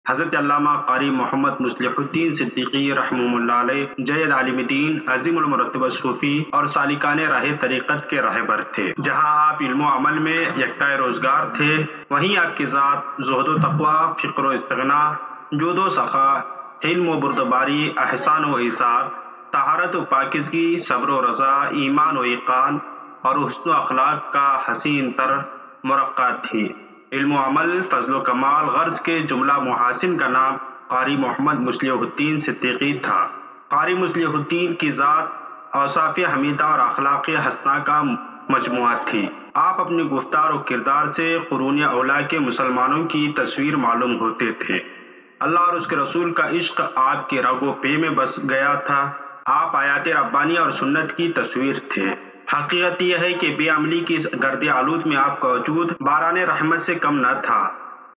Biography Qari Muhammad Muslehuddin Siddiqui | تعارف قاری محمد مصلح الدین صدیقی
Assalamu Alaikum

حضرت علامہ قاری محمد مصلح الدین صدیقی علیہ الرحمہ جیّد عالم دین، عظیم المرتبت صوفی، اور سالکان راہ طریقت کے رہبر تھے
Last Speech of Hadrat Allama Qari MuHammad MusleHuddin Siddiqui Qadiri Alaihir RaHma
Muhammad Muslehuddin Siddiqui (27 December 1918 – 23 March 1983) (Urdu محمد مصلح الدین صدیقى), was a preacher born in Nanded on India's Deccan Plateau.

Biography of Hazrat Allama Hafiz Qari Muhammad Muslehuddin Siddiqui Qadri Rizvi AlaiRehma

Copyright Declaimer